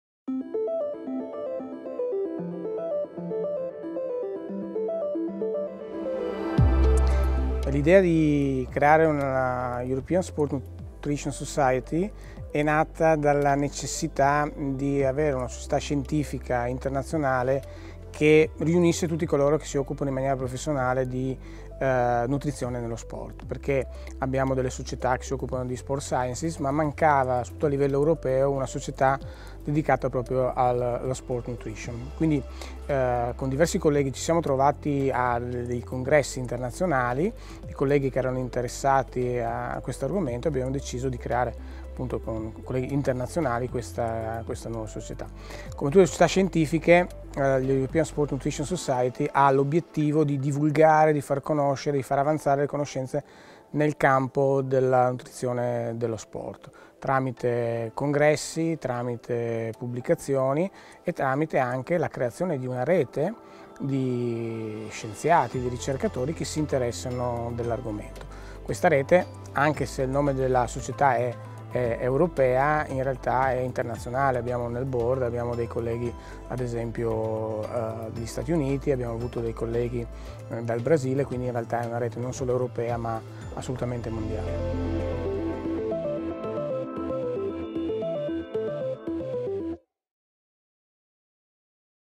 European Sport Nutrition Society: research and studies | ESNS
Antonio Paoli, ESNS Chairman and founding member describes ESNS goals and mission which promotes the scientific research and knowledge dissemination about sport nutrition.

For translation click on the "subtitles" icon to activate them, then click on the "settings" icon to select your language.